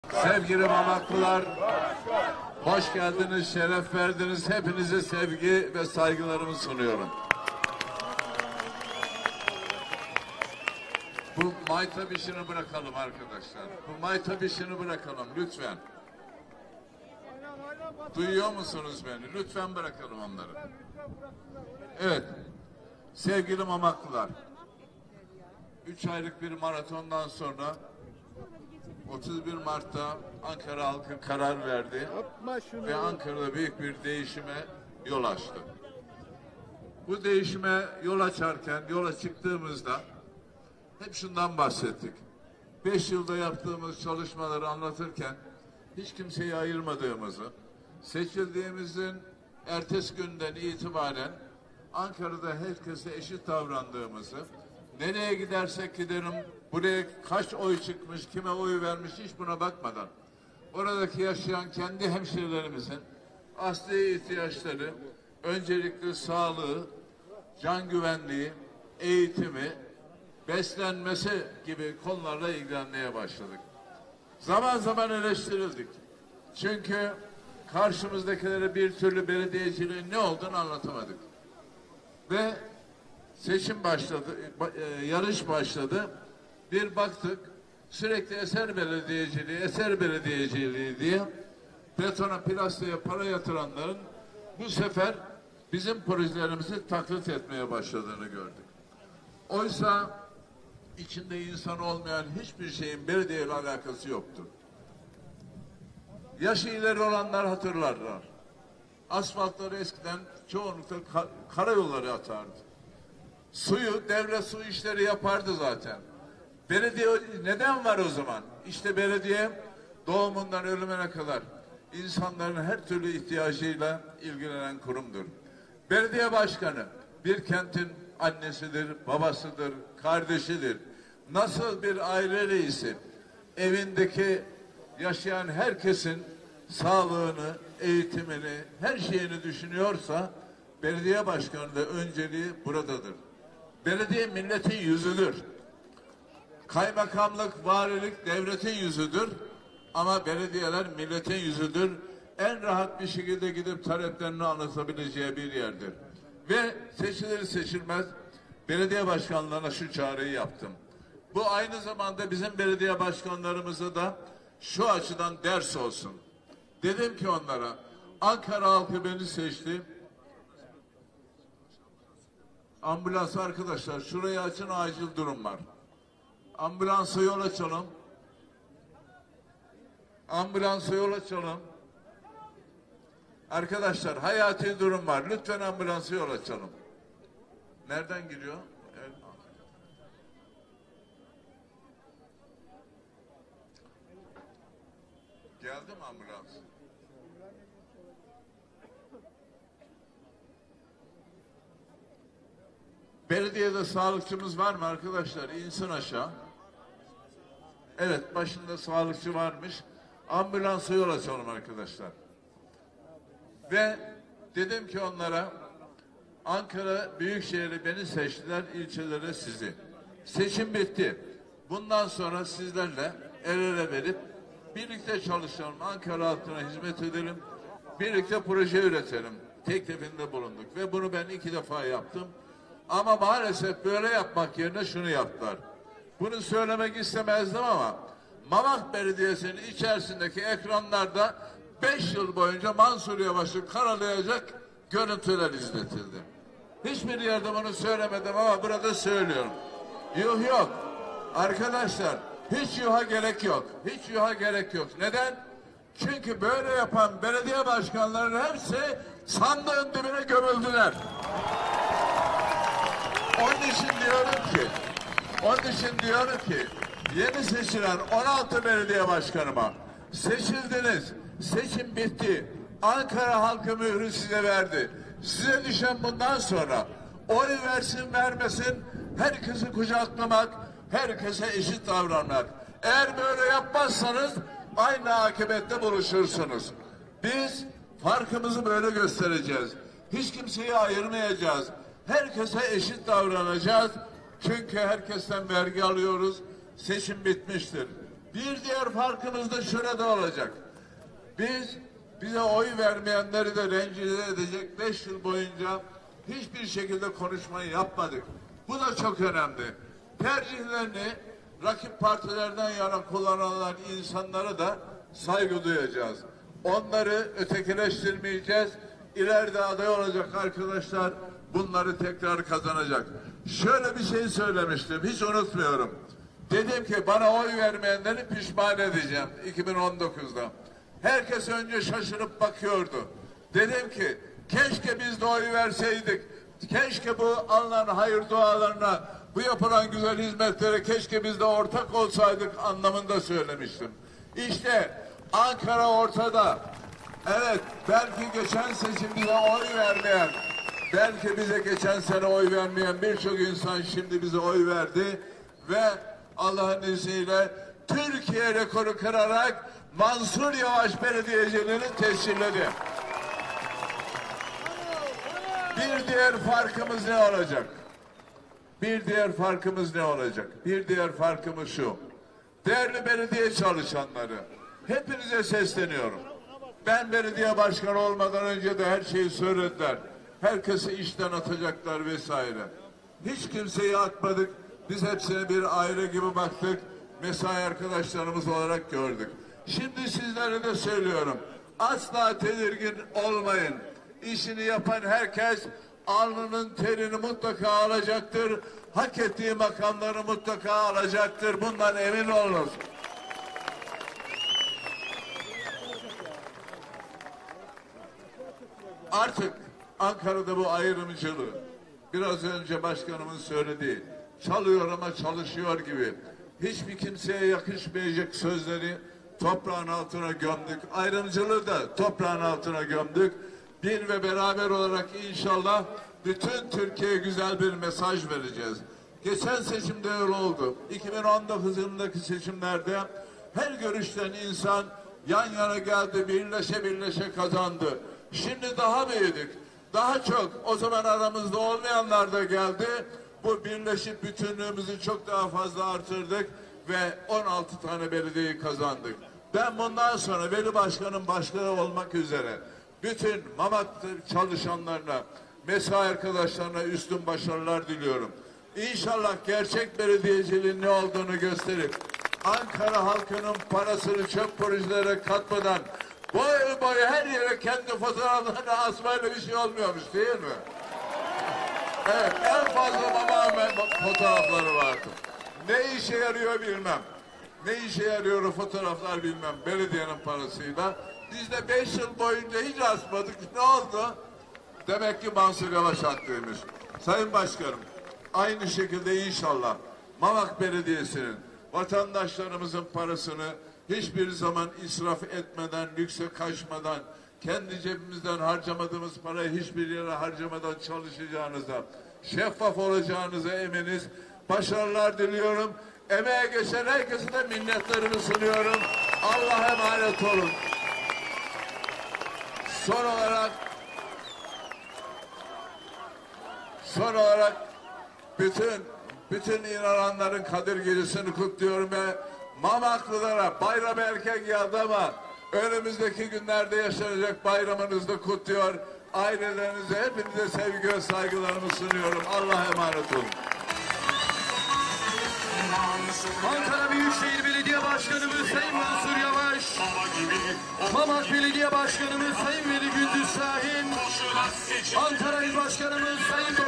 Mansur Yavaş'tan Mamak'ta zafer mitingi! AKP'nin kalesiydi CHP'ye geçti!
Ankara Büyükşehir Belediye Başkanı Mansur Yavaş, Mamak Belediyesi'nde devir-teslim törenine katıldı.